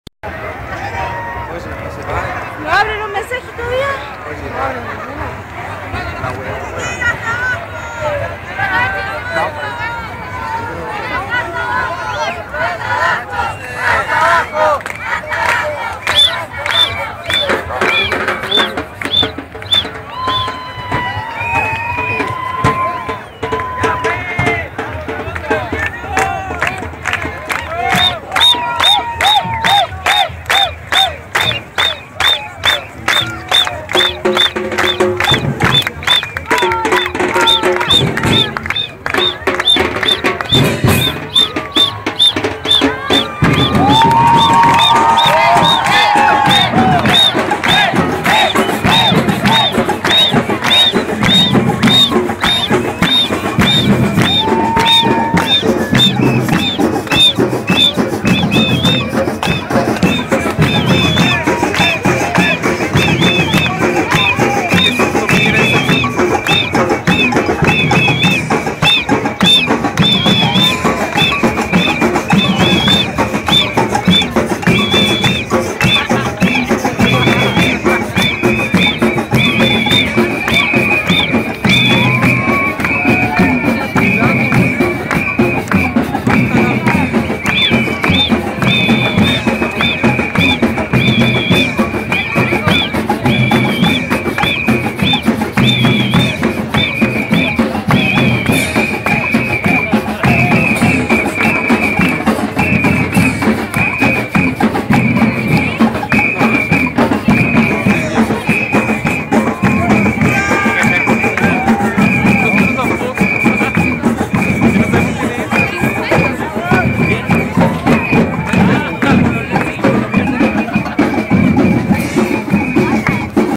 La marcha que no aparece en la TV
Mauricio Vega La marcha que no muestran en la tv... de plaza italia a moneda... cantos, gritos, buena onda, pero a la tv solo le importa el grupos de ahueo que se enfrentan con los carabineros o derechamente delincuentes q se aprovechan...